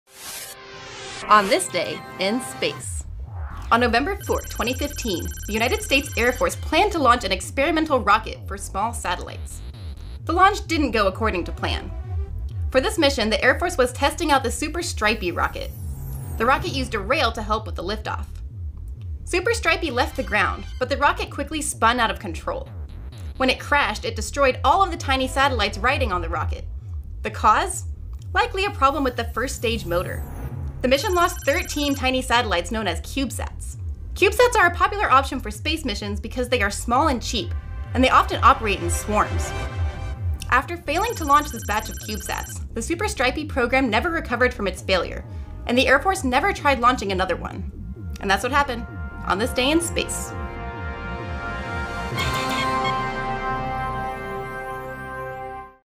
OTD in Space - Nov. 4: Experimental 'Super Strypi' Rocket Launch Fails
On November 4, 2015, the United States Air Force planned to launch an experimental rocket for small satellites. The launch didn't according to plan. For this mission, the Air Force was testing out the Super Strypi rocket. The rocket used a rail to help with the liftoff. Super Strypi left the ground, but the rocket quickly spun out of control. When it crashed, it destroyed all of the tiny satellites riding along the rocket. The cause? Likely a problem with the first stage motor. The mission lost 13 tiny satellites known as cubesats. Cubesats are a popular option for space missions, because they are small and cheap, and they often operate in swarms. After failing to launch this batch of cubesats, the Super Strypi program never recovered from its failure, and the Air Force never tried launching another one.